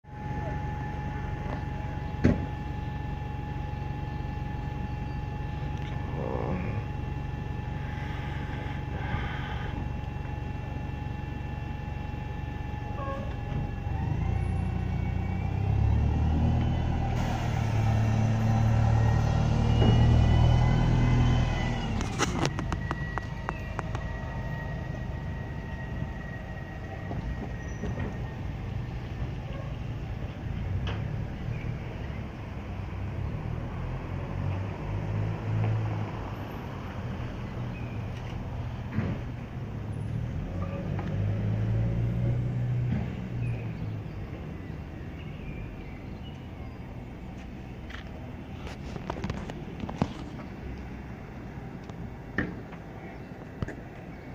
The silk Mills to gateway bus arrives at Taunton Musgrove Park hospital today. 1.5.2019.
Or my home two years ago